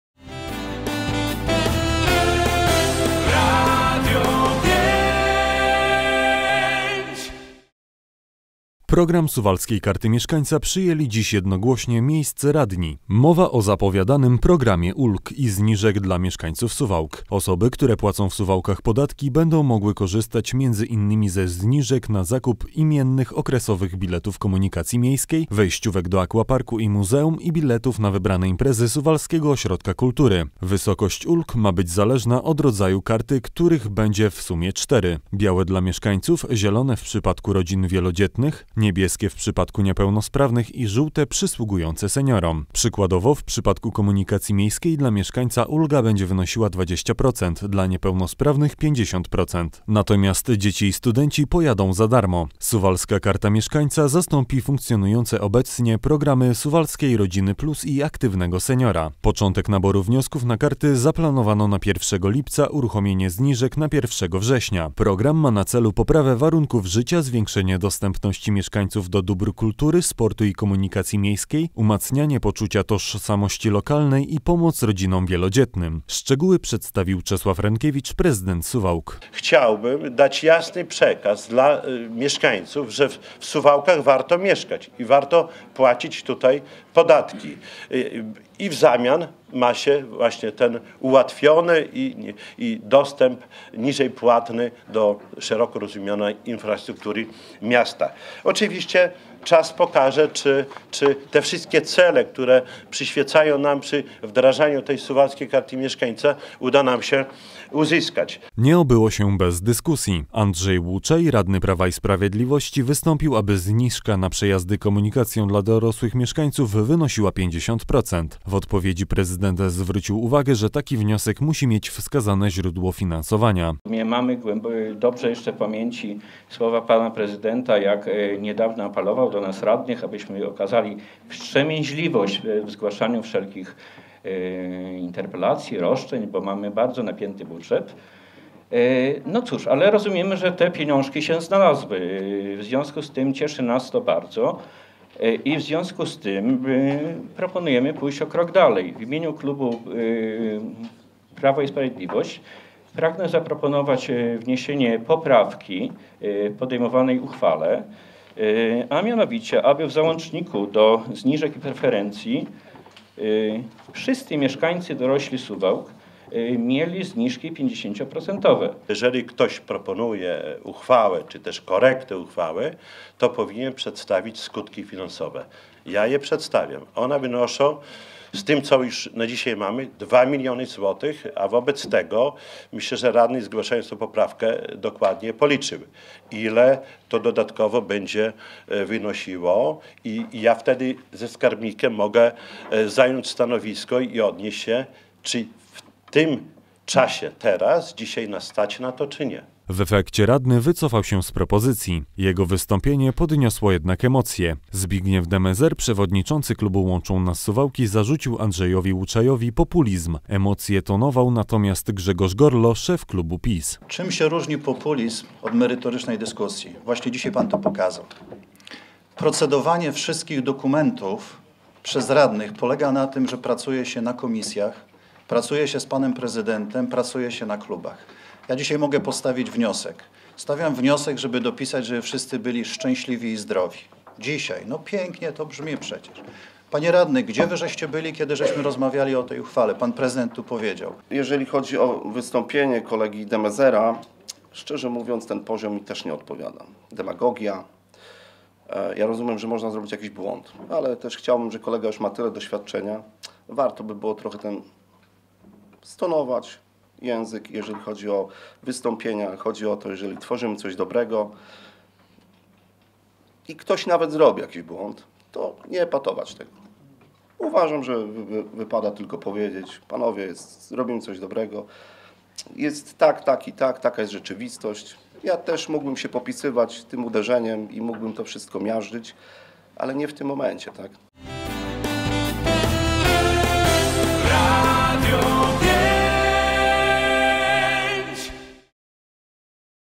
Będzie Suwalska Karta Mieszkańca
Program Suwalskiej Karty Mieszkańca przyjęli dziś jednogłośnie miejscy radni.Mowa o zapowiadanym pakiecie ulg i zniżek dla mieszkańców Suwałk. Osoby, które płacą w Suwałkach podatki, będą mogły korzystać między innymi ze zniżek na zakup imiennych, okresowych biletów komunikacji miejskiej, wejściówek do aquaparku i muzeum oraz biletów na wybrane imprezy Suwalskiego Ośrodka Kultury.  Wysokość ulg ma być zależna od rodzaju karty, których będzie w sumie cztery: białe dla mieszkańców, zielone w przypadku rodzin wielodzietnych, niebieskie w przypadku niepełnosprawnych i żółte, przysługujące seniorom.  Przykładowo, w przypadku komunikacji miejskiej, dla mieszkańca ulga będzie wynosiła 20 procent, dla niepełnosprawnych 50 procent, natomiast  dzieci i studenci pojadą za darmo. Suwalska Karta Mieszkańca zastąpi funkcjonujące obecnie  programy Suwalskiej Rodzina Plus i Aktywnego Seniora. Początek naboru wniosków na karty zaplanowano na 1 lipca, uruchomienie zniżek na 1 września. Program ma na  celu poprawę warunków życia, zwiększenie dostępności mieszkańcow do dóbr kultury, sportu i komunikacji miejskiej, umacnianie poczucia tożsamości lokalnej i pomoc rodzinom wielodzietnym. Szczegóły przedstawił Czesław Renkiewicz, prezydent Suwałk.
Nie obyło się bez dyskusji. Andrzej Łuczaj, radny Prawa i Sprawiedliwości wystapił, aby zniżka na przejazdy komunikacją dla dorosłych mieszkańców wynosiła 50 procent. W odpowiedzi prezydent zwrócił uwagę, że taki wniosek musi mieć wskazane źródło finansowania. W efekcie radny wycofał się z propozycji. Jego wystąpienie podniosło jednak emocje.  Zbigniew De-Mezer, przewodniczący klubu "Łączą nas Suwałki" zarzucił Andrzejowi Łuczajowi populizm. Emocje tonował natomiast Grzegorz Gorlo, szef klubu PiS.